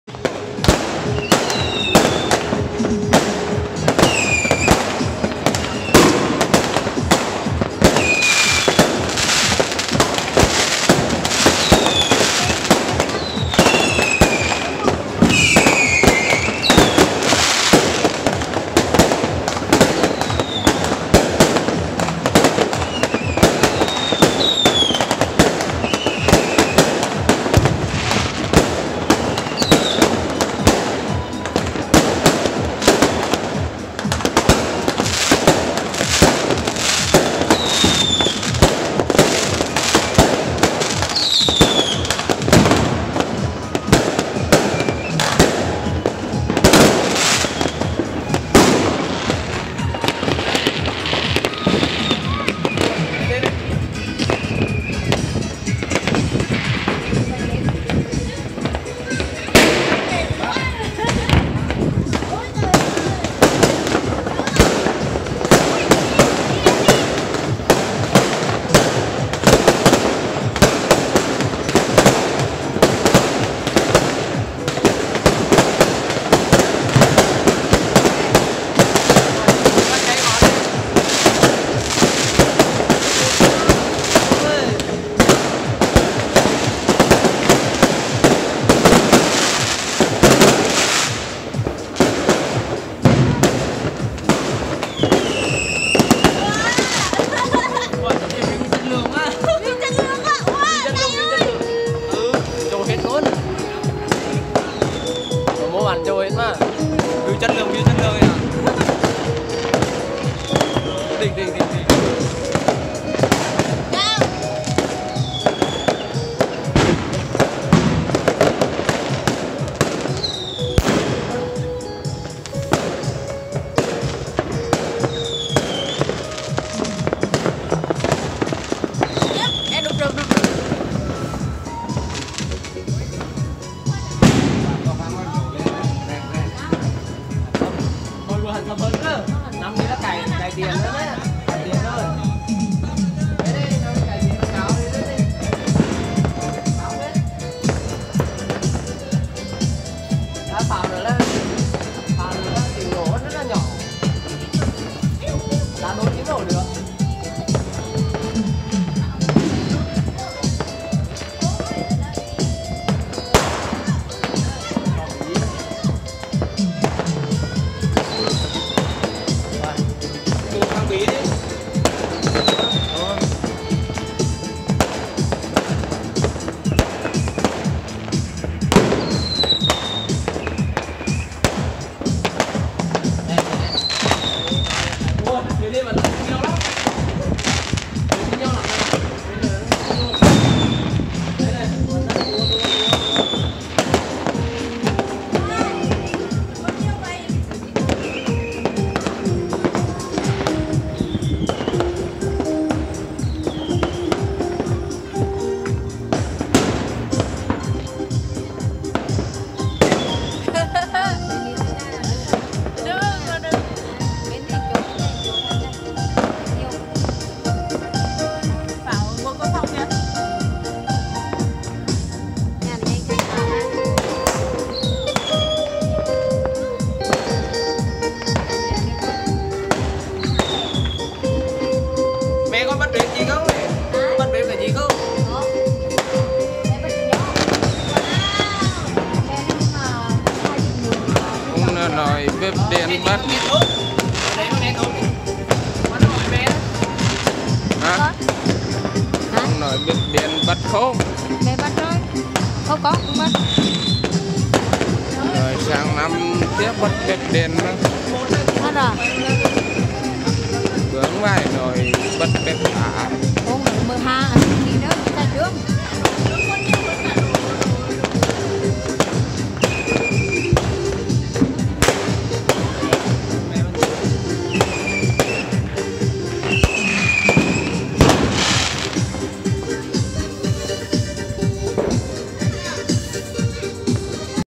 Làng tôi đầy đủ các loại pháo : pháo quốc phòng, Thailand, 6x6, 7x7, loại 9 ống to và pháo trứng
pháo hoa